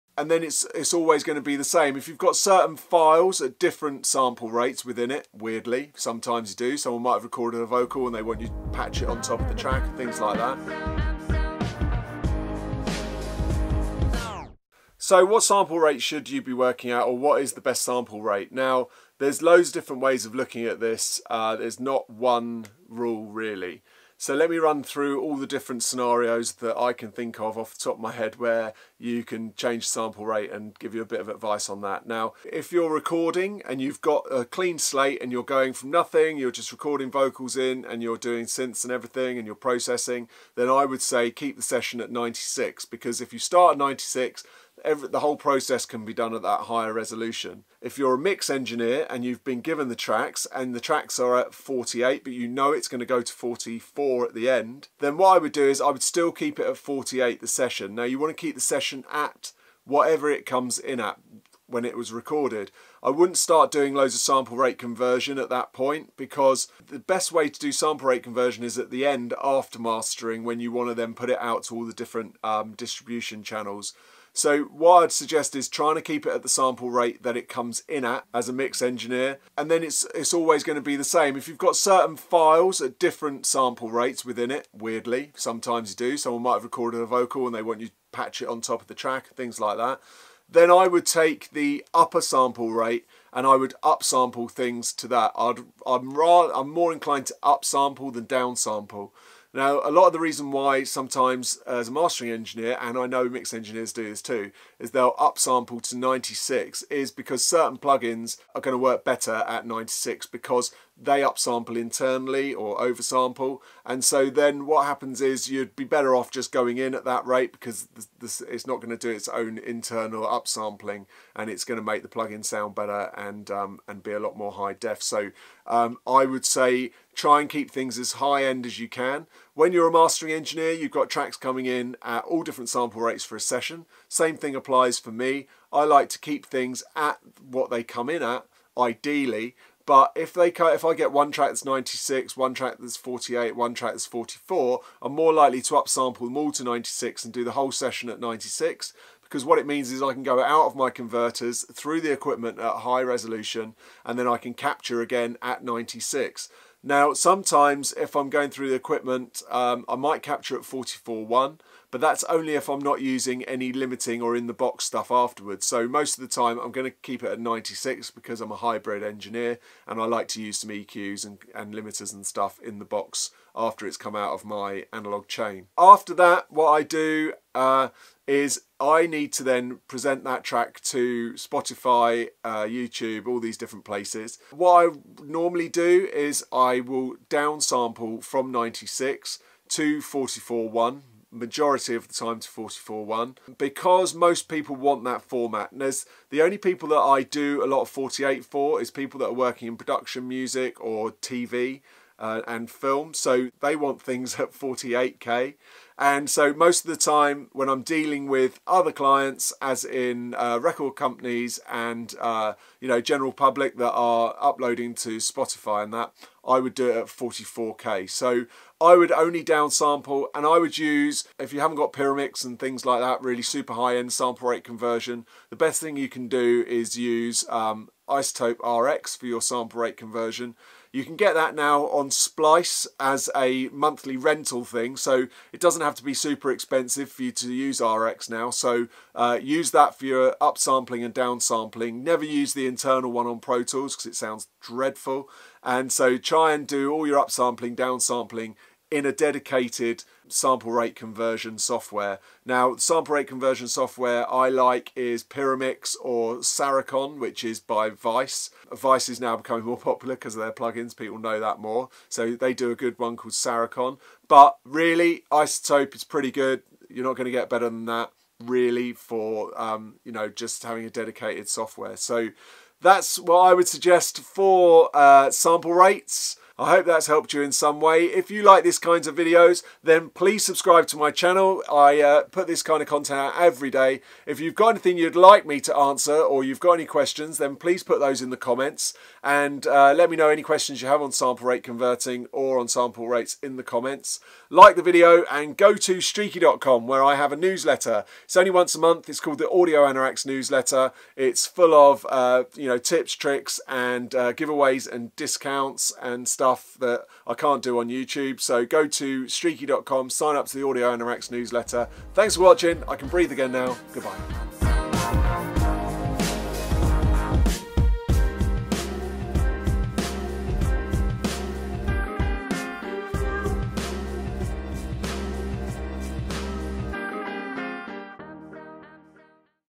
CAN YOU GUESS WHAT"S THE BEST SAMPLE RATE? | Streaky.com
When it comes to sample rates everybody seems to have an option, well these are the sample rates I'm using daily for mastering.

▶ LEARN MASTERING WITH STREAKY - “I Will Teach You To Master” Mastering Course

Streaky Ltd, Pinewood Studios, Iver Heath, SL0 0NH

So what sample rate should you be working out or what is the best sample right now? There are loads of different ways of looking at this. There's not one rule really, so let me run through all the different scenarios that I can think of, off the top of my head where you can change sample rate and give you a bit of advice on that. Now, if you're recording, and you've got a clean slate, and you're going from nothing, you're just recording vocals in, and you're doing since and everything and you're processing. Then I would say keep the session at 96 because if you start at 96, the whole process can be done at that higher resolution.

 If you're a mix engineer, and you've been given the tracks, and the tracks are at 48, but you know it's going to go to 44 at the end, then what I would do is I would still keep it at 48 the session. Now you want to keep the session at whatever it comes in at when it was recorded. I wouldn't start doing loads of sample rate conversion at that point because the best way to do sample rate conversion is at the end after mastering when you wanted to put it out to all the different distribution channels… so what I'd suggest is trying to keep it at the sample rate that it comes in at as a mix engineer and then it's always going to be the same if you've got certain files or different sample rates within it. Weirdly sometimes you do someone might have recorded a vocal, and they weren't just patch it on top of the track and things like that.

 Then I would take the upper sample rate, and I would up sample things to that. I'm more inclined to up sample than down sample. Now, a lot of the reason why sometimes as a mastering engineer and I know mix engineers do this too, is that they'll up sample to 96 is because certain plugins are going to work better at 96.  They up sample internally or over sample and so then what happens is you'd be better off just going in at that rate because this is not going to do its own internal up sampling and it's going to make the plugin sound better and be a lot more high def. So, I would say try and keep things as high end as you can. When you are a mastering engineer, you've got tracks coming in at all different sample rates for a session. Same thing applies for me.

I like to keep things at what they come in at ideally, but if I get one track that's 96, one track, that's 48, one track that's 44, I'm more likely to up sample more to 96 and do the whole session at 96.  What that means is I can go out of my converters through the equipment at high resolution and then I can capture again at 96. Now, sometimes if I'm going through the equipment, I might capture it 44 one but that's only if I'm not using any limiting or in the box stuff afterwards. So most of the time I'm going to keep it a 96 because I'm a hybrid engineer, and I like to use EQ's and limiters and stuff in the box after it's come out of my analogue chain. After that, what I do is I need to then present that track to Spotify, YouTube, all these different places.

 What I normally do is I will down sample from 96 to 44 one majority of the time to 44 one because most people want that format. And there's the only people that I do a lot of 48 for is people that are working in production, music, or TV and film. So they want things at 48K. And so most of the time when I'm dealing with other clients as in record companies and general public that are uploading to Spotify and that I would do it at 44K. So, I would only down sample and I would use, if you haven't got Pyramix and things like that really super high in sample rate conversion, the best thing you can do is use iZotope RX for your sample rate conversion.

You can get that now on Splice as a monthly rental thing. So it doesn't have to be super expensive for you to use RX now. So, use that for your up sampling and down sampling. Never used the internal one on Pro Tools because it sounds dreadful. And so try and do all your up sampling down sampling in a dedicated sample rate conversion software. Now sample rate conversion software, I like is Pyramix or Saracon which is by Weiss. Weiss is now becoming more popular because of their plugins. People know that more. So they do a good one called Saracon, but really iZotope, it's pretty good.